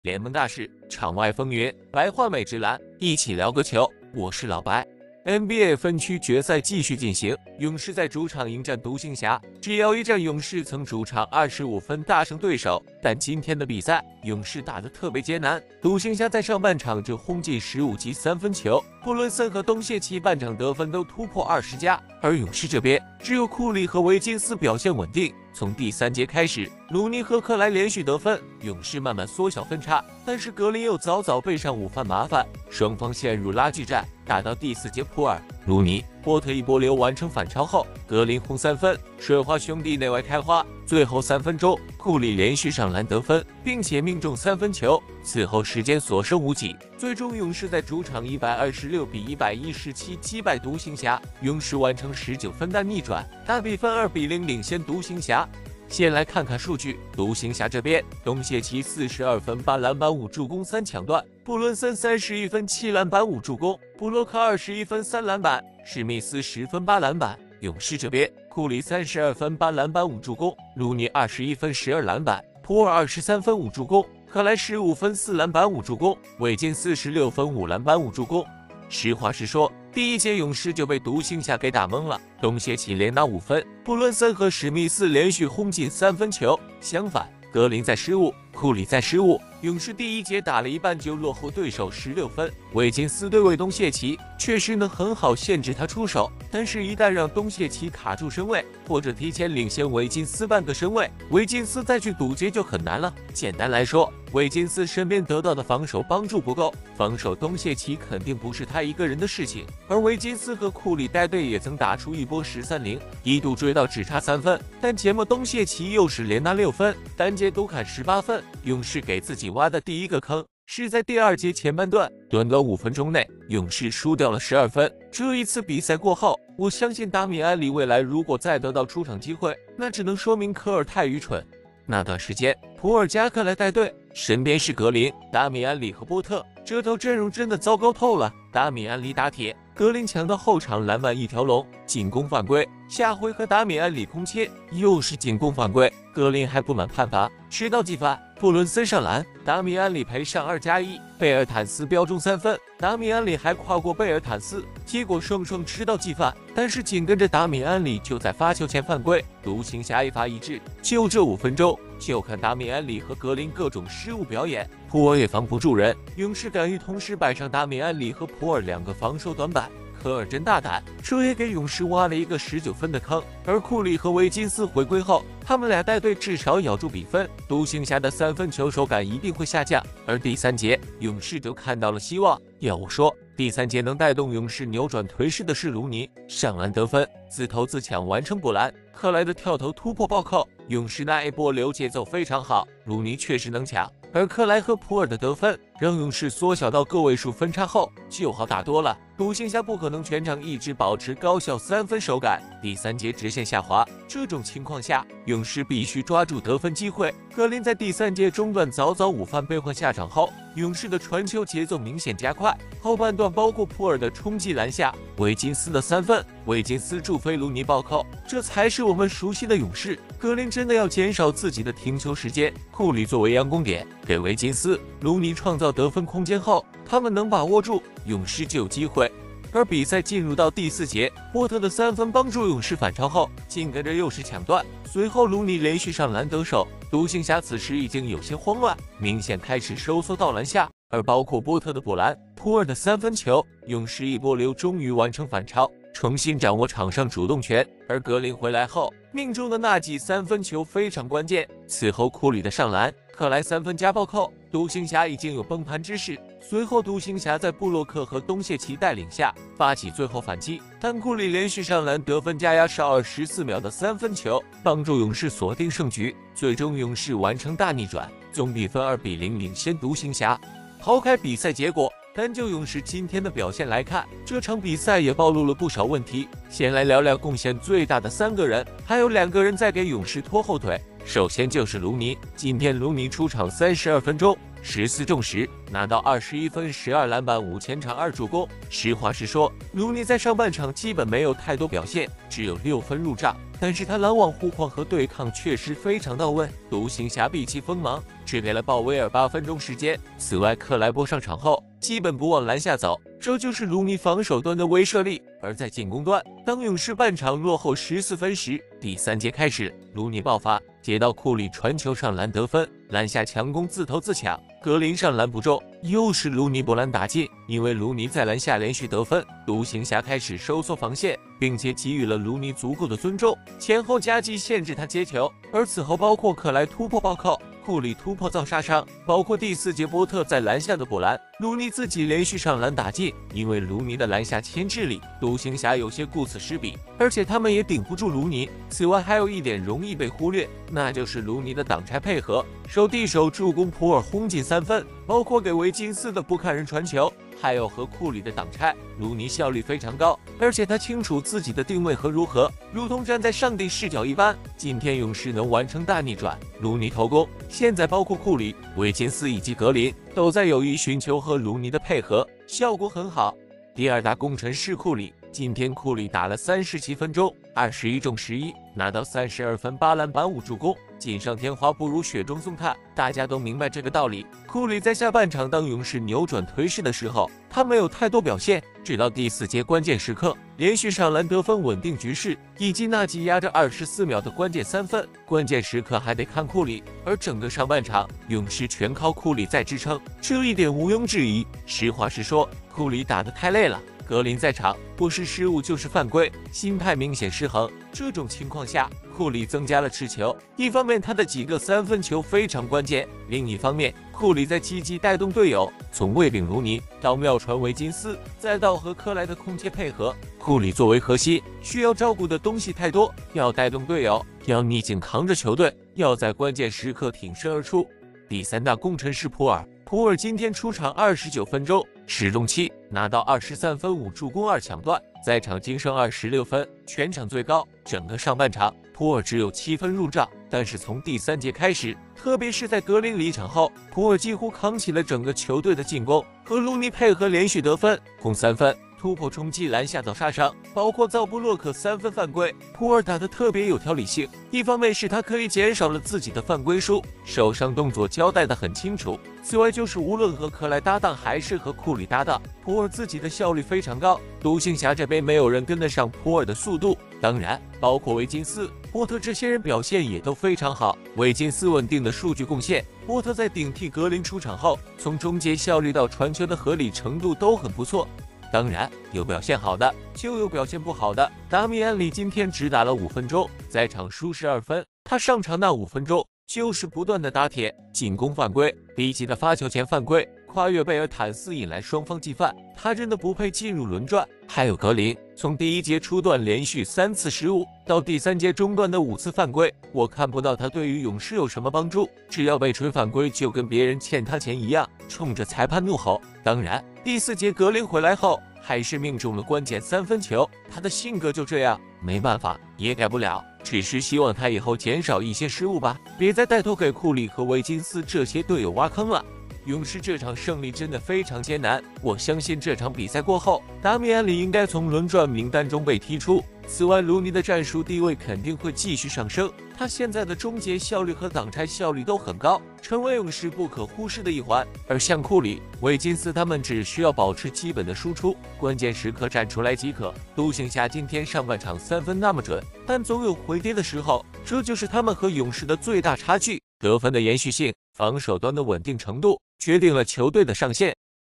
2-0！ 總決賽在招手！ 勇士完成19分大逆轉！ 庫裡32+6記三分化身死神！ 東77空砍42分！ 6人得分上雙！ 盧尼21+12立大功！ 達米安李學AI跨人吃技犯！ 格林表現該批！NBA#
2-0！ 總決賽在招手！ 勇士完成19分大逆轉！ 庫裡32+6記三分化身死神！ 東77空砍42分！ 6人得分上雙！ 盧尼21+12立大功！ 達米安李學AI跨人吃技犯！ 格林表現該批！